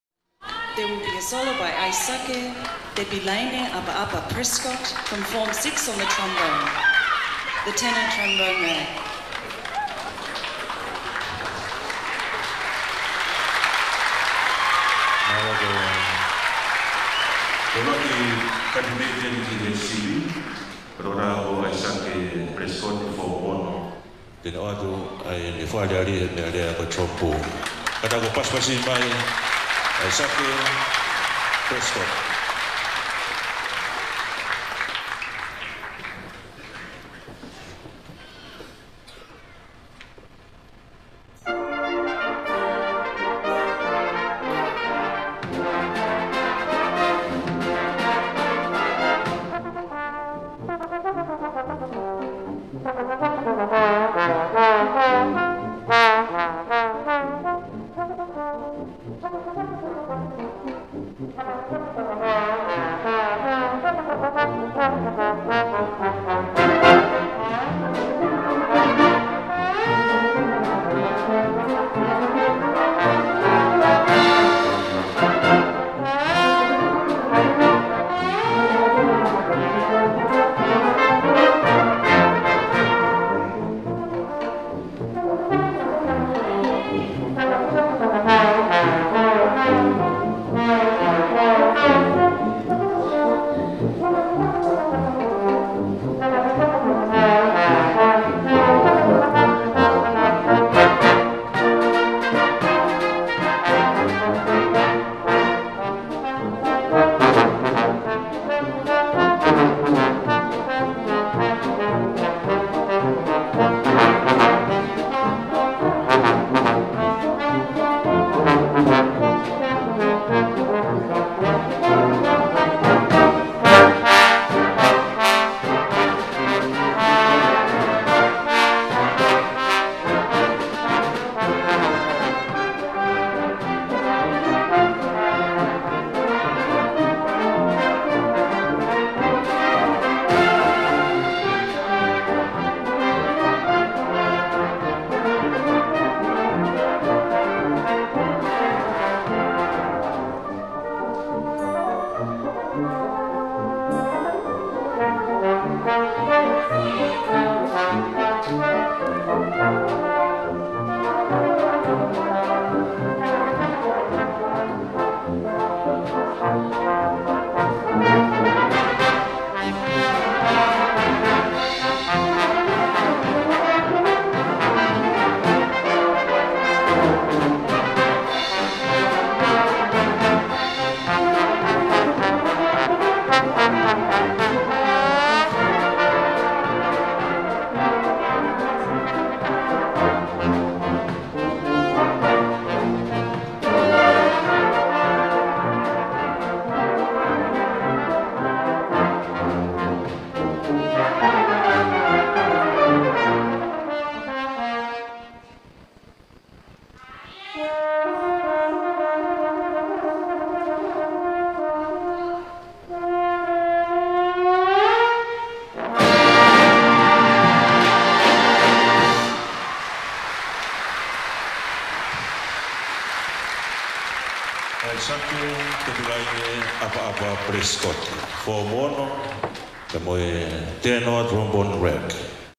Tupou College Music festival 2020 - Solo Trombone 'Aisake Prescott
Source: Tongan Broadcasting (Live on Facebook and YouTube
Location: Tupou College Toloa